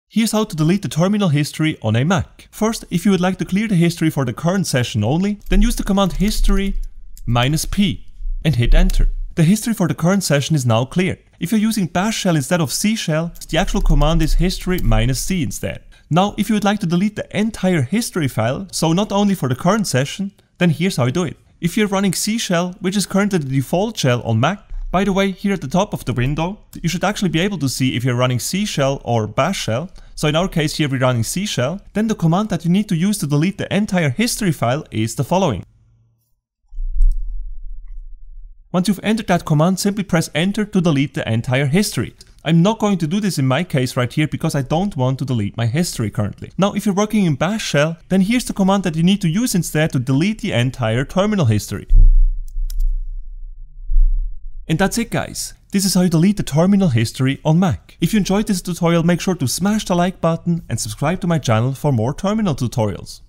How to Delete the Terminal History on Mac
In this video, I will show you how to delete the terminal history on mac. We will have a look at how to delete the history for the current terminal session as well as how to delete the history for the entire terminal (= all sessions).